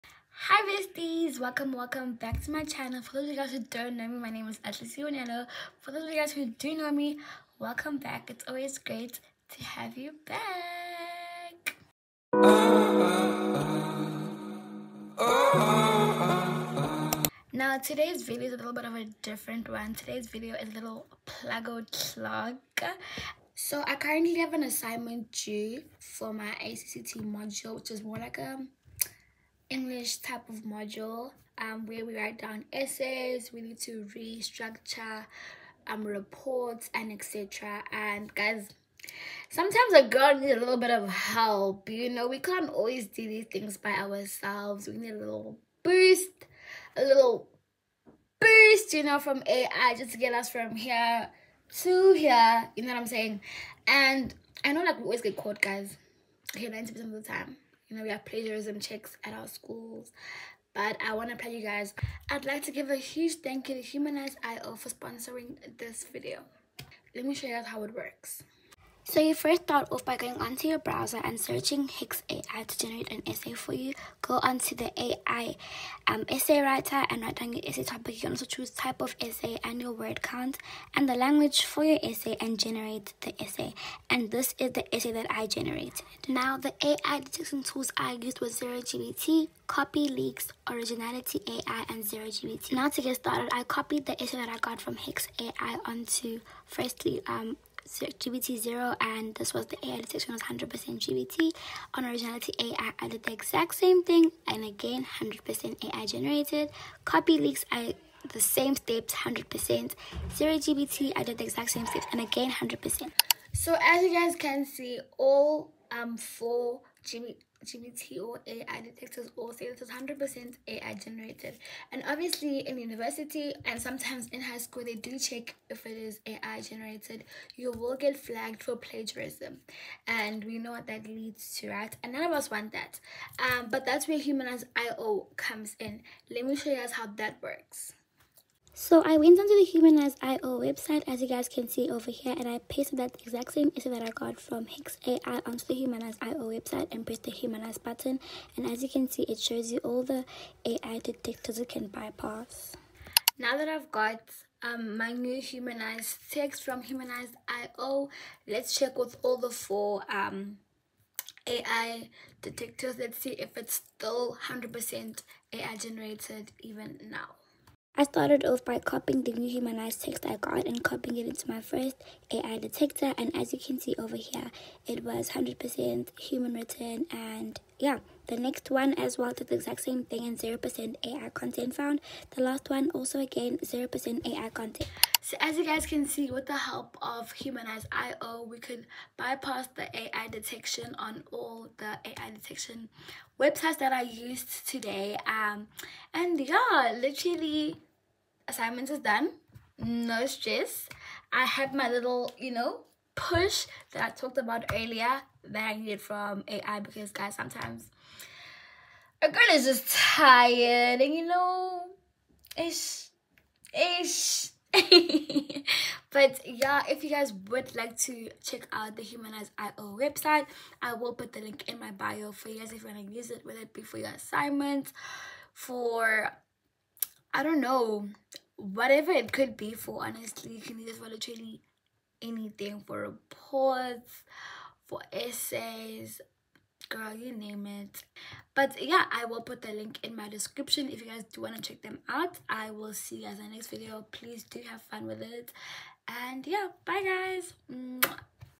STUDY DAYS WITH Humanize.io | Humanize AI Text and Bypass AI Detection
🚀 In this video, we dive deep into the fascinating world of essay writing! 📚✍️ Learn how to create undetectable essays using the cutting-edge power of artificial intelligence, specifically with Humanizeio. 🤖💡
📌 In this video, you'll discover:
- 🖊️ How to get started with Humanizeio
- ⚙️ The features and capabilities of Humanizeio
- 👨‍🏫 Strategies to ensure authenticity and avoid plagiarism detection
🎉 Whether you're a student looking for an academic edge or simply fascinated by the power of AI, this video is for you! 🎓🌟